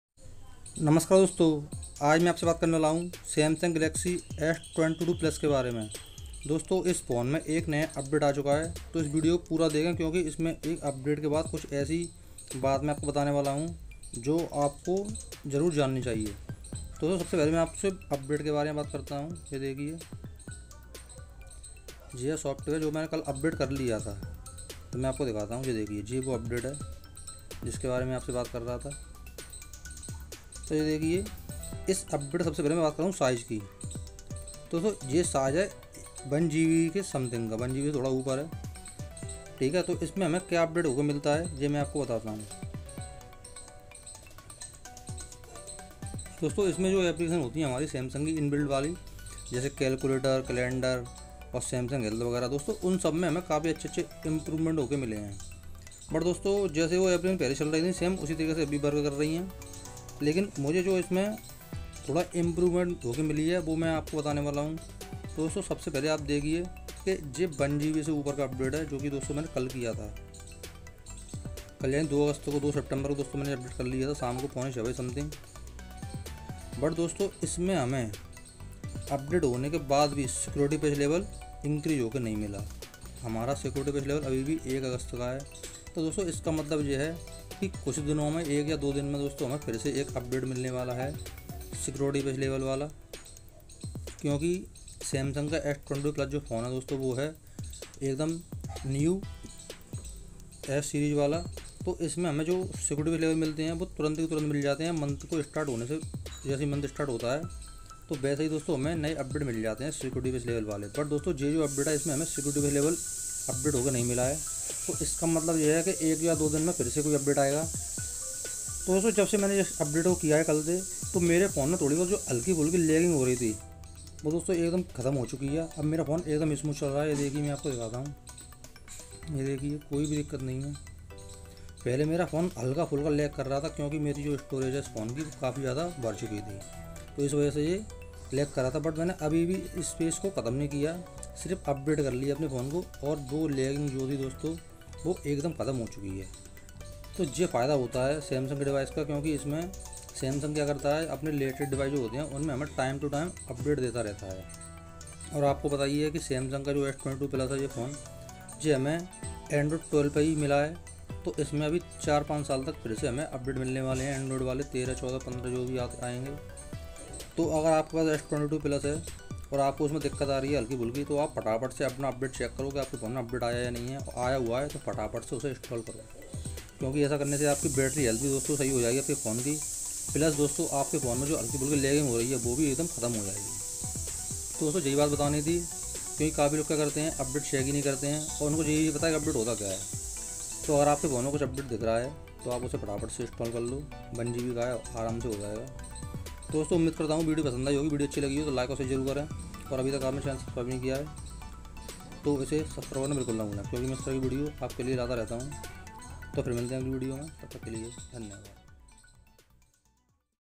Samsung Galaxy s22 Plus New Software Update Rolling Out September 2022🔥🔥
S22 plus me new update mil gya h esme kya Features Include huye h ye sv ap es video me janoge